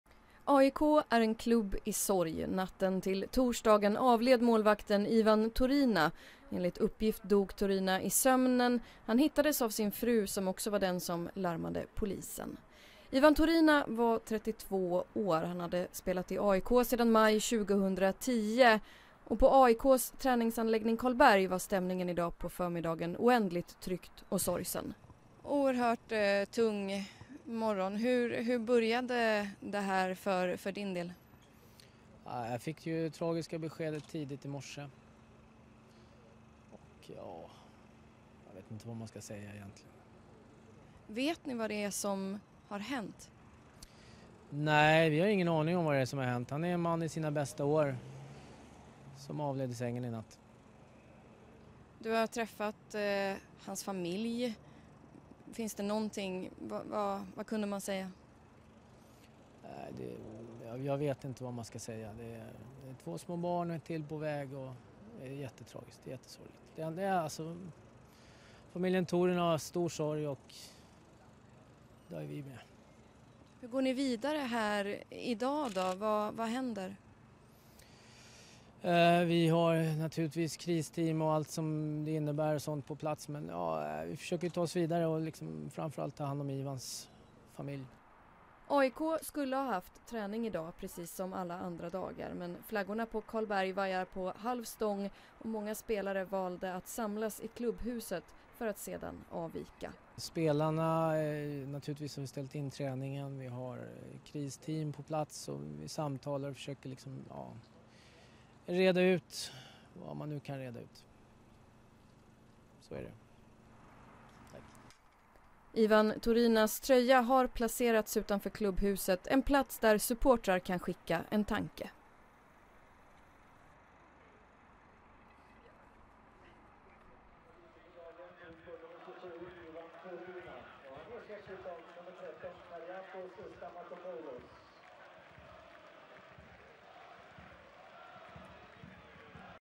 AIK : Ivan turina är död.
Ett klibb som kom ut när Ivan Turina hade död.